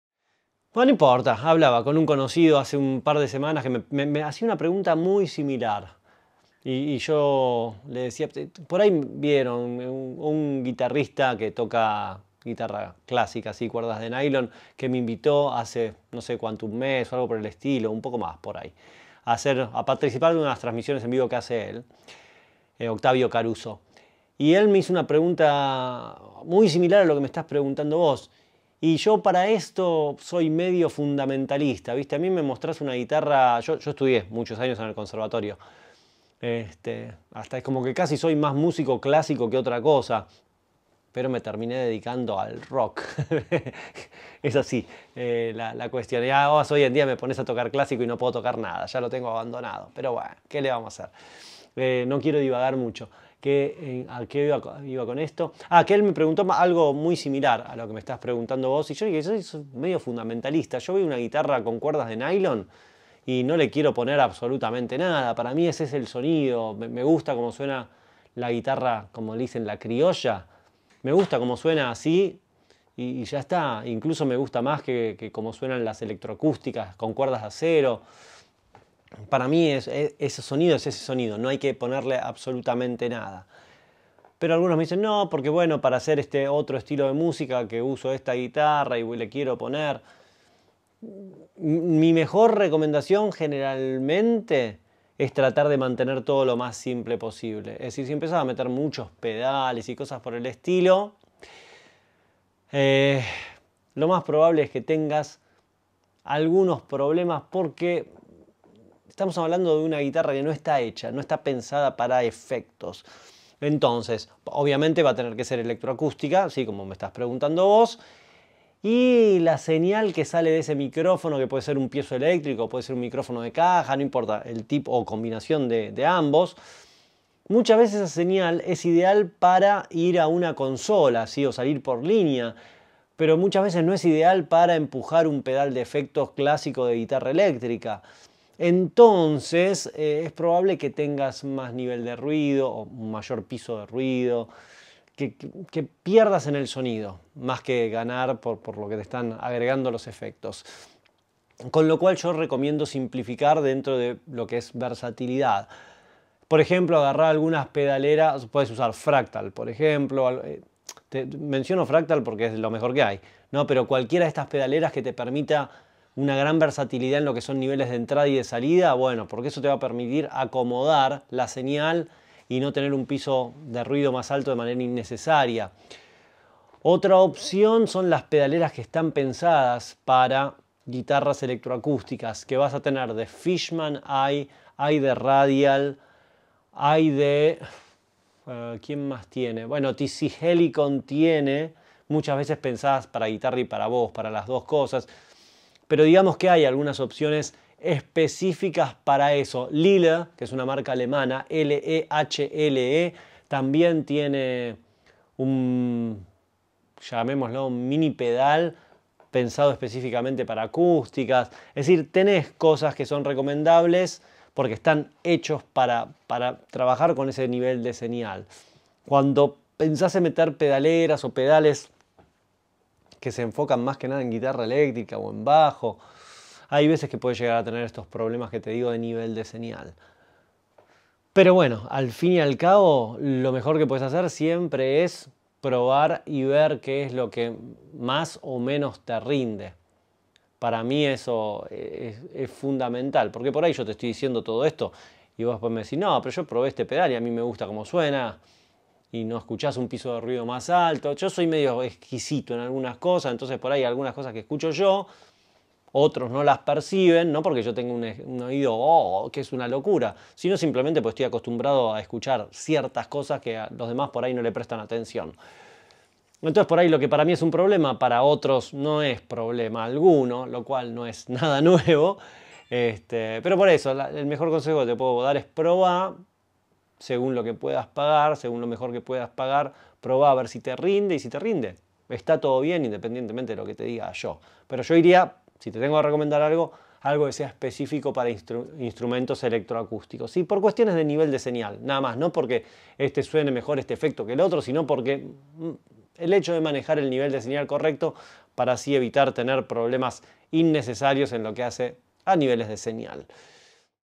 Recomendamos efectos para electroacústica
Tocás con electroacústica y no podés vivir sin efectos??? Acá te recomendamos las mejores opciones para sacar el mayor provecho posible de tu audio.